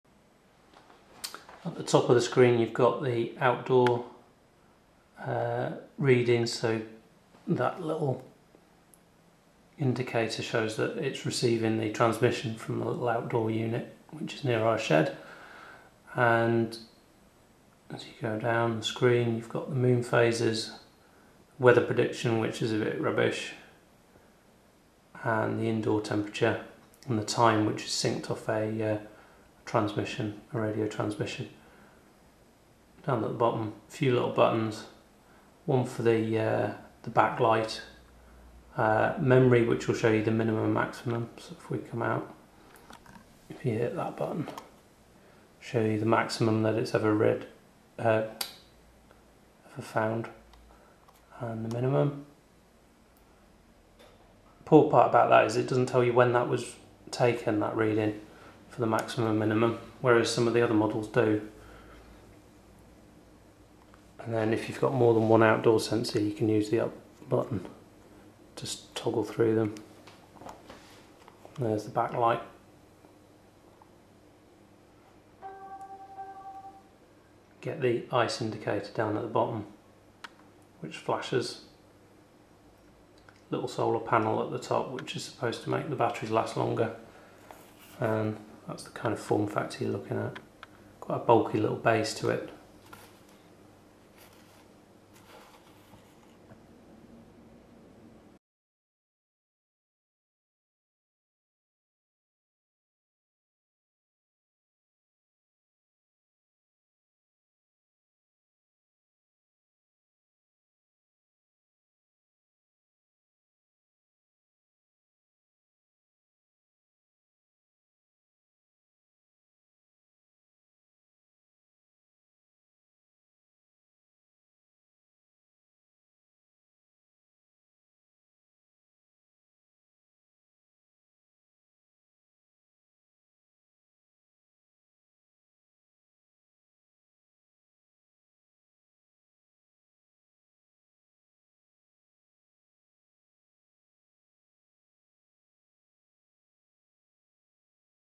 Oregon Scientific BAR808 Weather Station - Quick Look in HD
This is a quick look at the Oregon Scientific Weather Station, model number BAR808. It's a good gadget which we've found really useful.. more useful than we imagined really. It's kept in the kitchen near the kettle so we see it throughout the day.

I'd have said the only 2 minus points are the min/max readings aren't date-stamped, and we had to move it from it's original position in the kitchen due to some interference which made the digital readout show dashes instead of the temperature. We thought this was a fault but I found that we should try moving it on some forums.